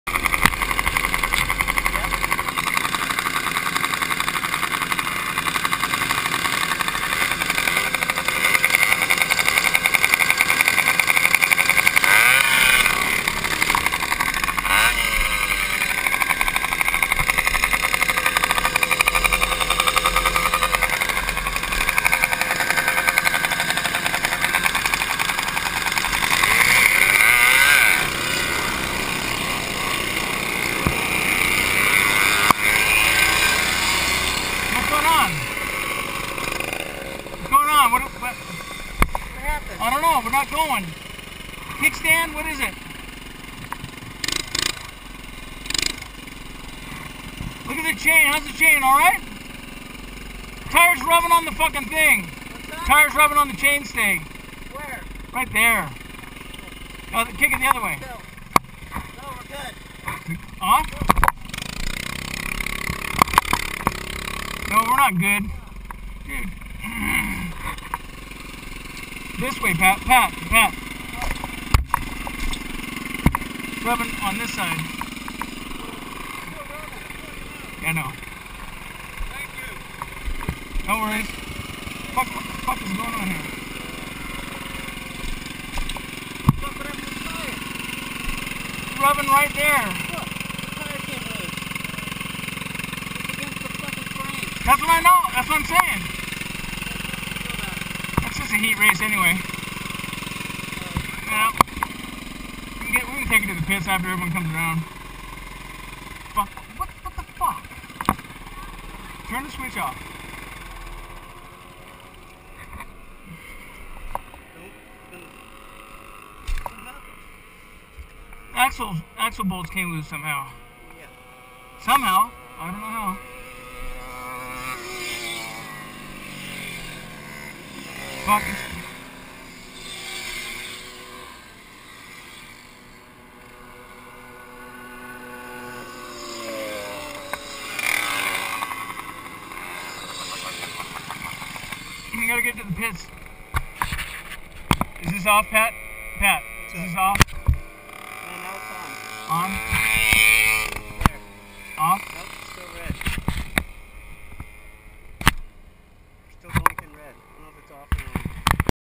Tandem breaks rear axle GOPR0386.MP4
Start of Heat race when the tandem breaks the rear axle right off the starting line. This was a major let down as the tandem was ready to rock this track and Pat didn't get to race. The beast was running fast and strong. oh well, hopefully next time.